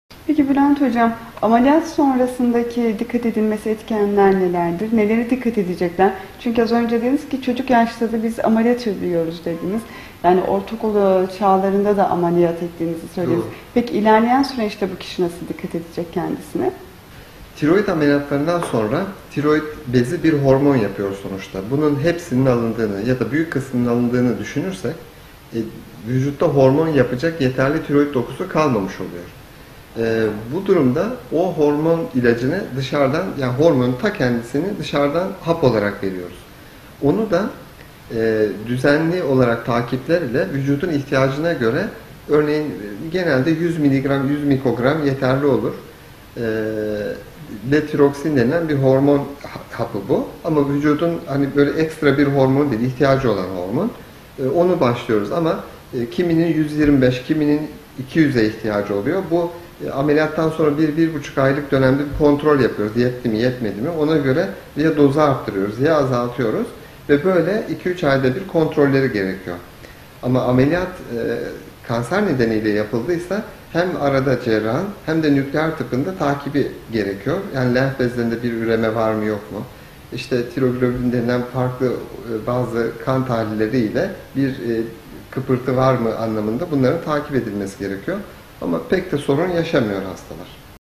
Ameliyat Sonrası Gelişmeler
Avrasya Hastanesi Genel Cerrahi Uzmanı Op. Dr. Bülent Öztürk '' Ameliyat Sonrası Gelişmeler '' hakkında detaylı bilgiler veriyor.